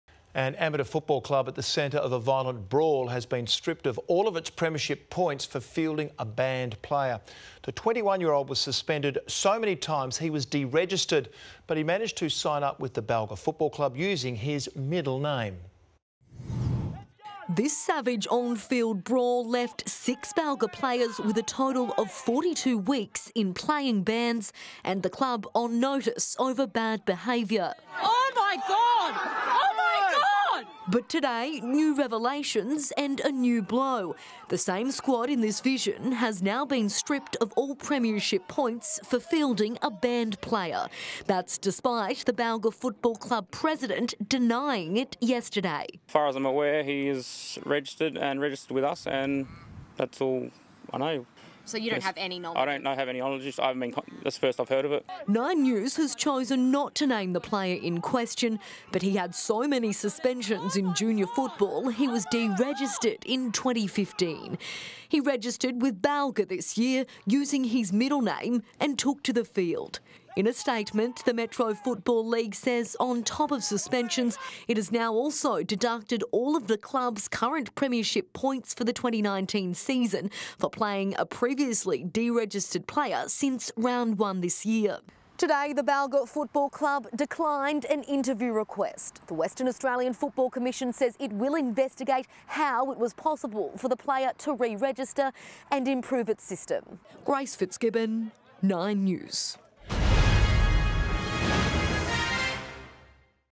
Balga Footy Ban | 9 News Perth
An amateur football club at the centre of a violent brawl has been stripped of all its premierships points for fielding a banned player.